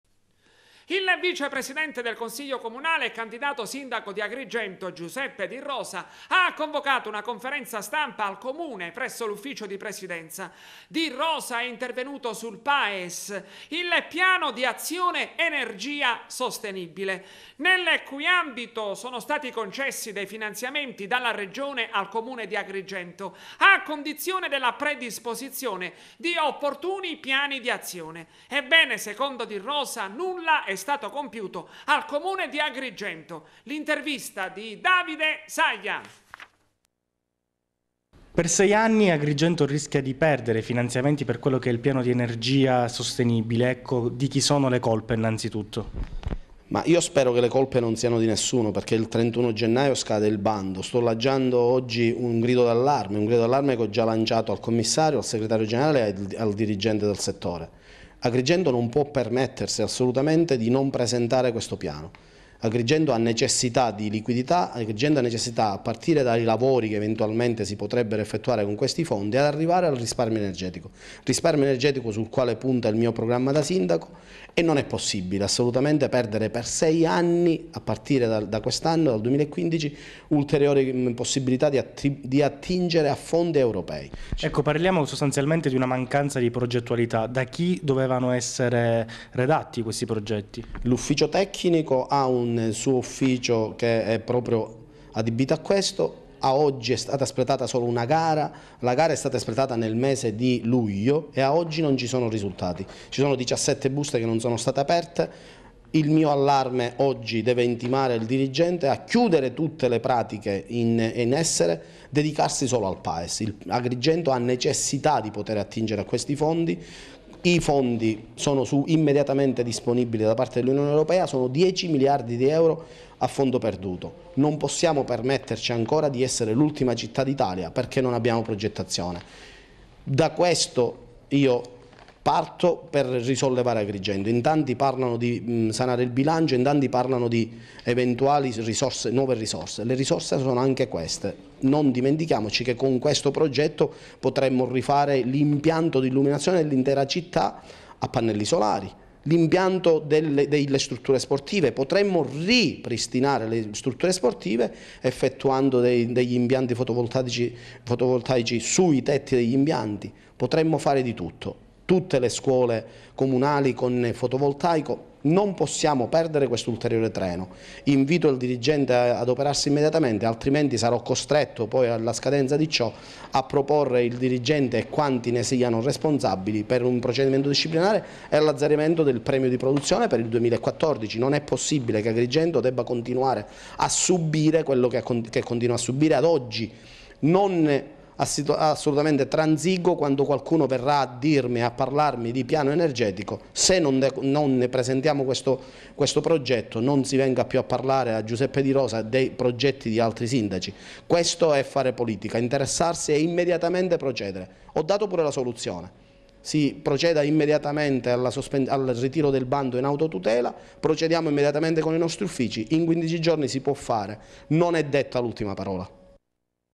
Teleacras - Di Rosa sul Paes
Il vice presidente del Consiglio comunale e candidato sindaco di Agrigento, Giuseppe Di Rosa, ha convocato una conferenza stampa al Comune, presso l’ Ufficio di Presidenza. Di Rosa è intervenuto  sul Paes, che è il Piano d’azione energia sostenibile, nel cui ambito sono stati concessi dei finanziamenti dalla Regione al Comune di Agrigento a condizione della predisposizione di opportuni piani di azione. Ebbene, secondo Di Rosa nulla è stato compiuto al Comune di Agrigento. L’ intervista di Davide Saia.